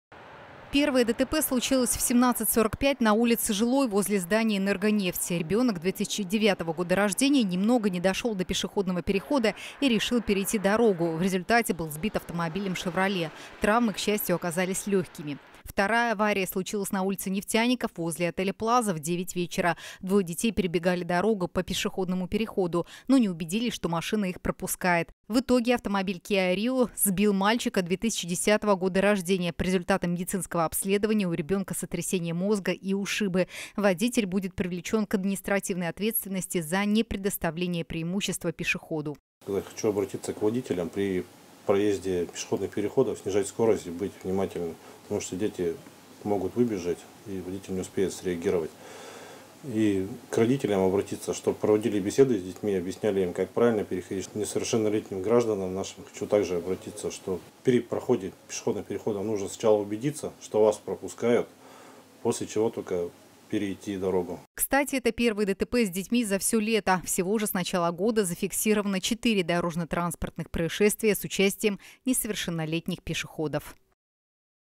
2 ребенка были сбиты на дороге 17 августа
Два ребенка пострадали накануне вечером в дорожно-транспортных происшествиях. Оба переходили дорогу и были сбиты автомобилями.